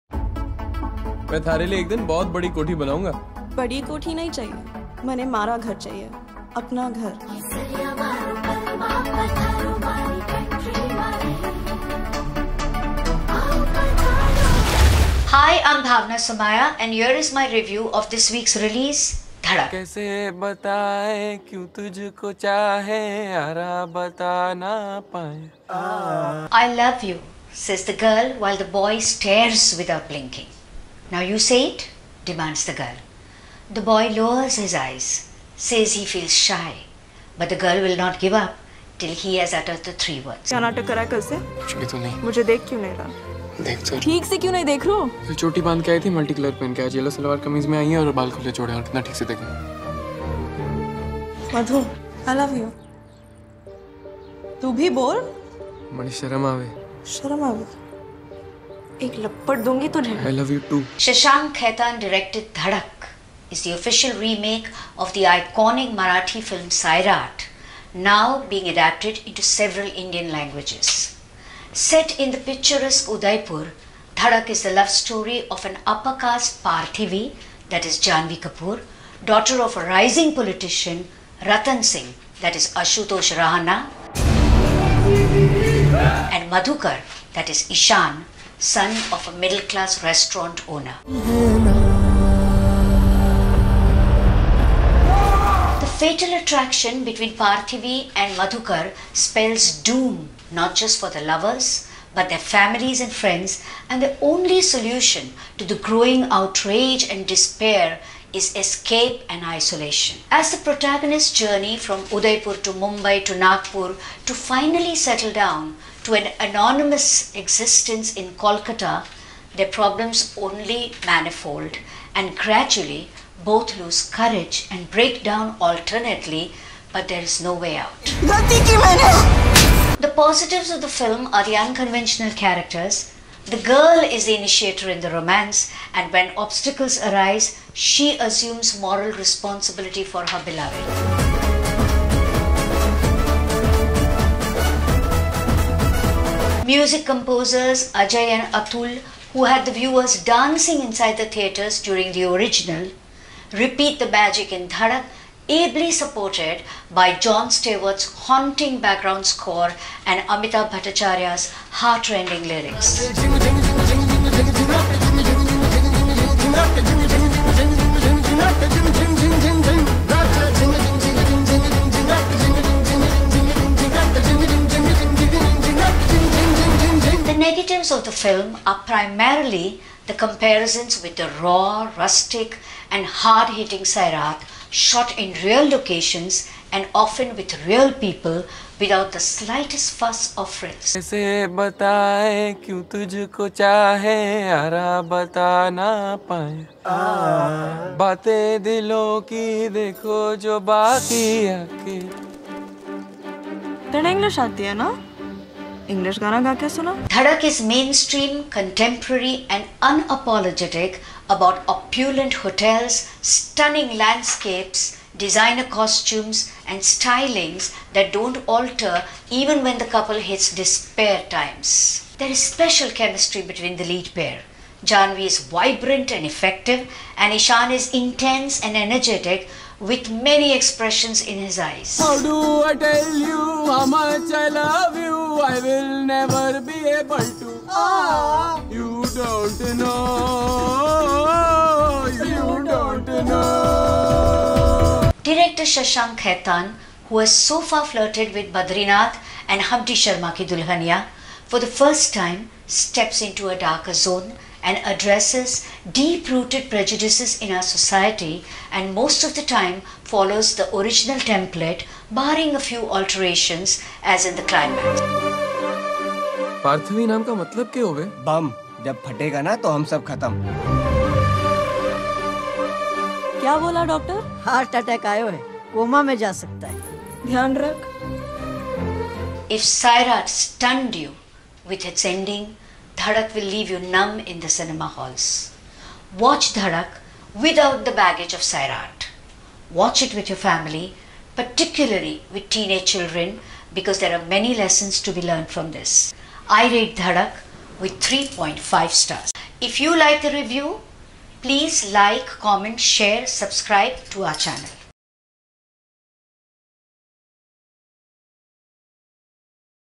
DHADAK MOVIE REVIEW I JANHVI KAPOOR I ISHAAN KHATTER I BHAWANA SOMAAYA
WATCH DHADAK WITHOUT THE BAGGAGE OF SAIRAT.

Bhawana Somaaya reviews movie Dhadak featuring Ishaan Khattar and Janhvi Kapoor.